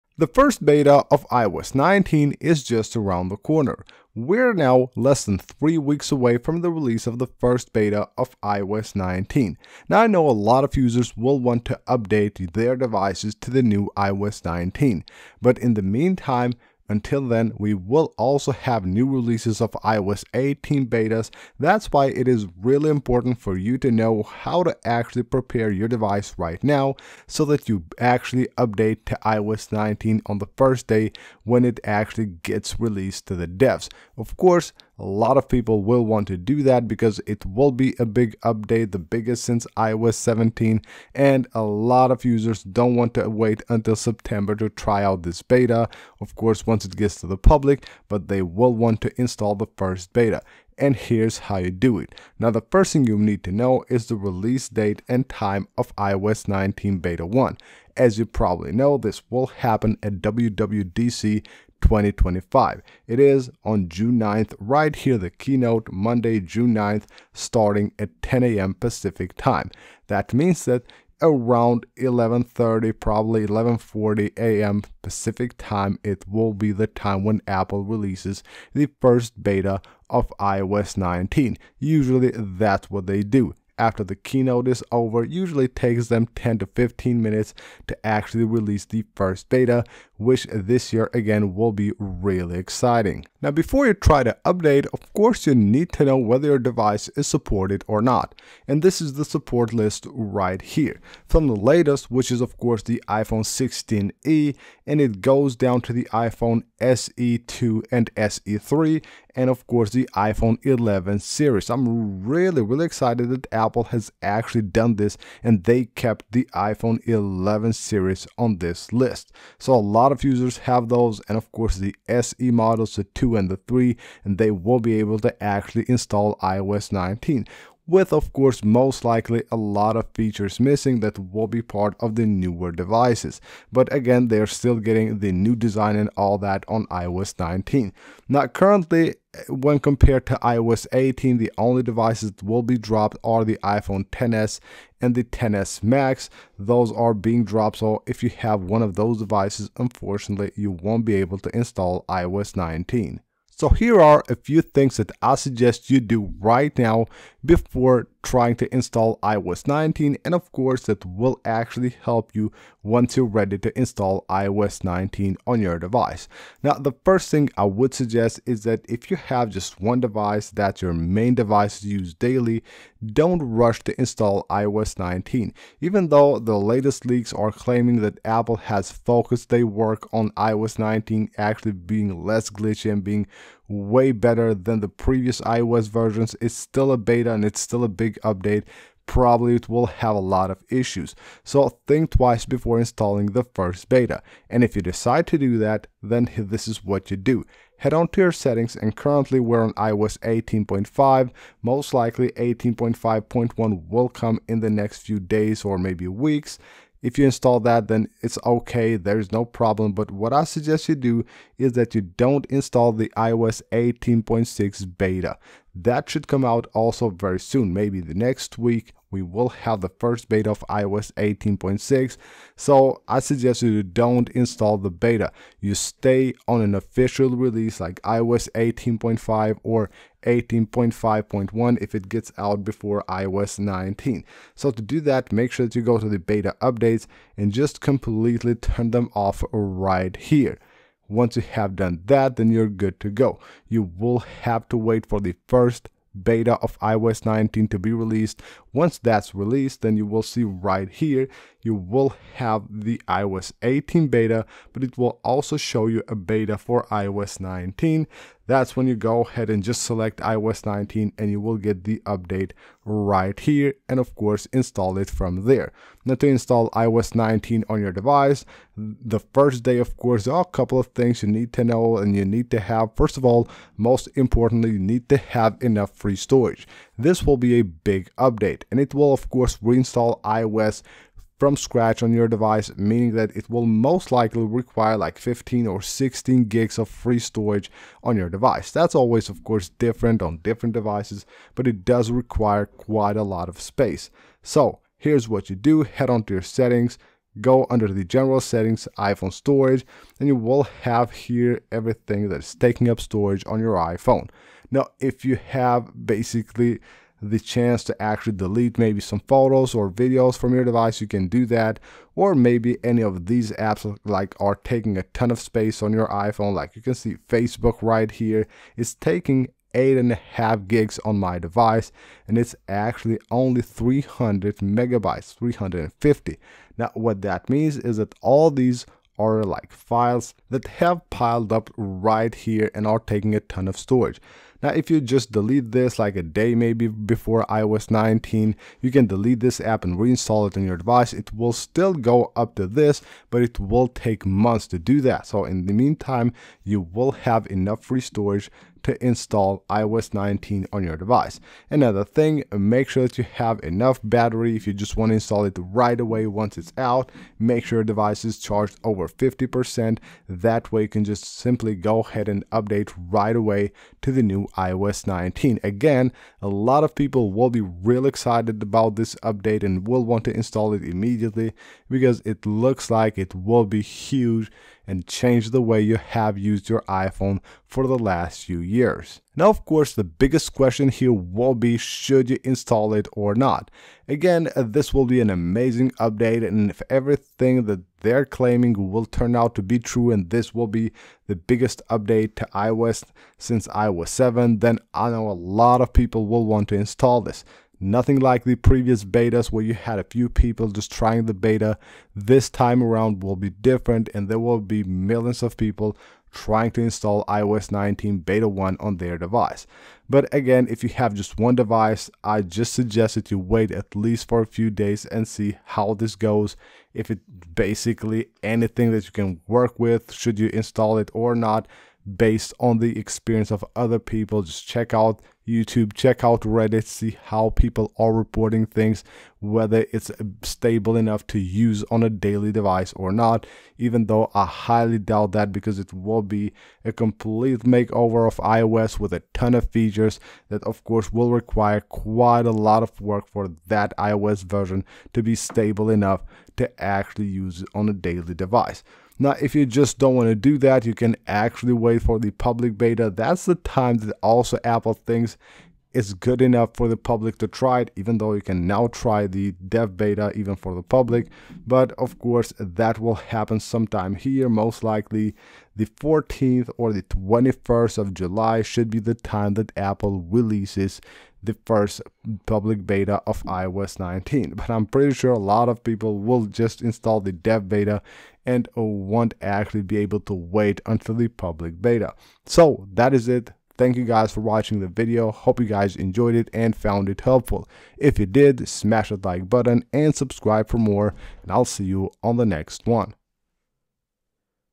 Prepare Your iPhone to UPDATE to iOS 19 Beta !
iOS 19 beta 1 is now days away from being released with a lot of new features, a redesign and a lot more changes, so in this video I will show you how to prepare your iPhone to update to the new iOS 19 beta.

Release 0:00
Devices 1:33
Do This 2:33
Should You Update 6:19